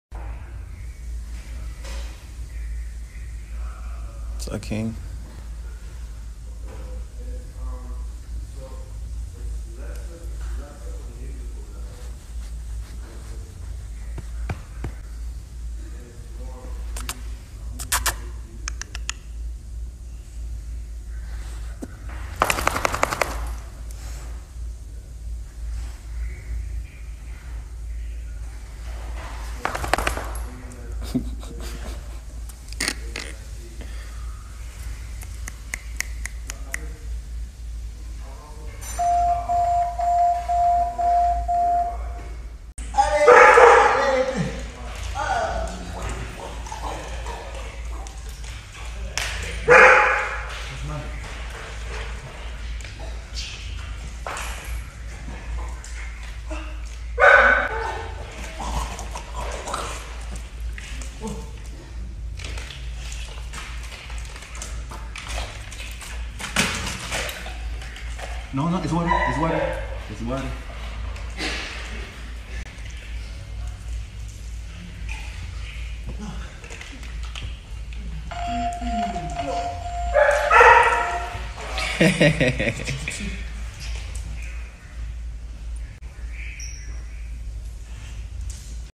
Les Twins spending time with Jason Derulo’s dog!
Via... lestwinsoff...IGS♥️♥️♥️ Los Angeles, California